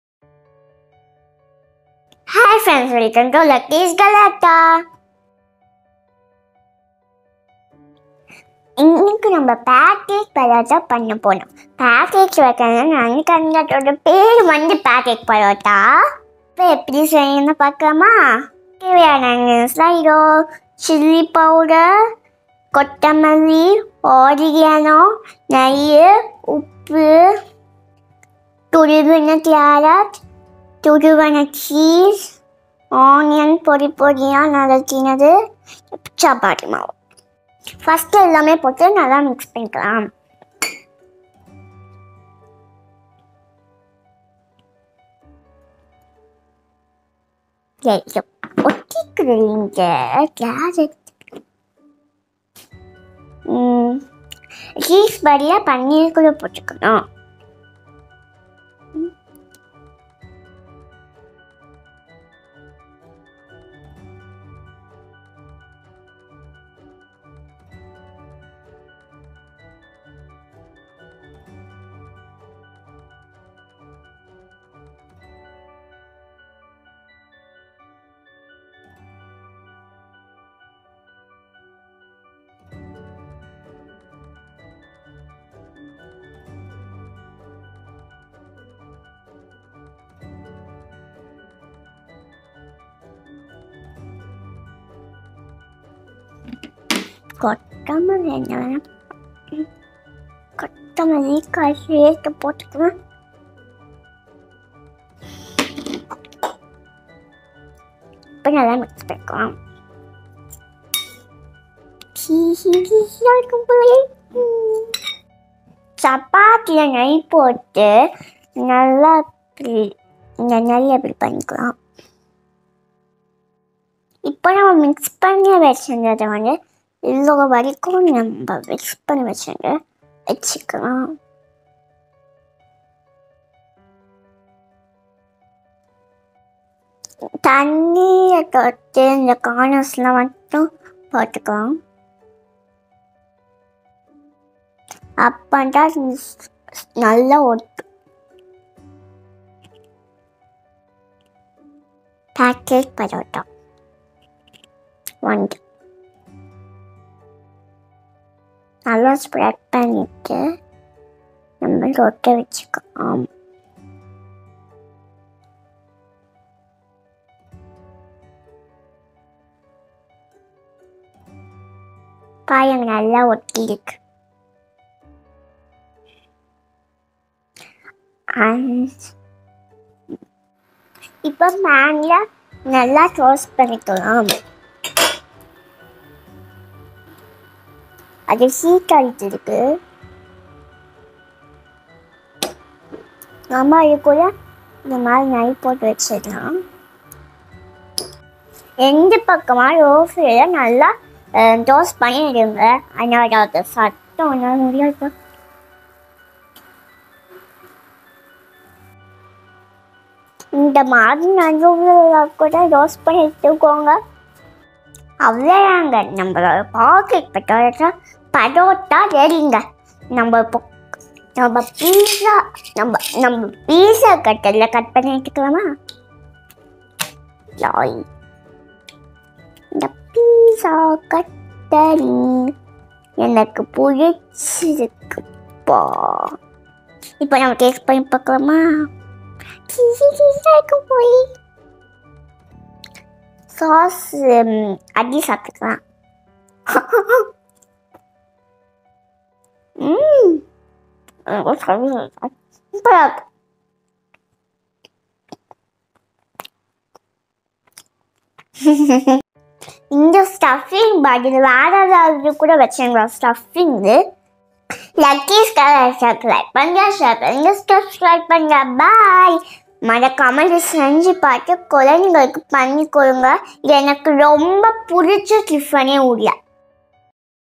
Pocket Parota in Tamil | Cheese Paratha in tamil | How to prepare pocket parota
Pocket Parota
Ingredients
Chapati Dough
Grated carrot
Grated Cheese
Onions
Coriander leaves
Salt
Chilli powder
Oregano

Mix all ingredients together. Place the mixture in one half of the chapati, apply water in all corners of the chapati and close it. In low flame toast both sides of the chapati. Pocket parrot is ready now.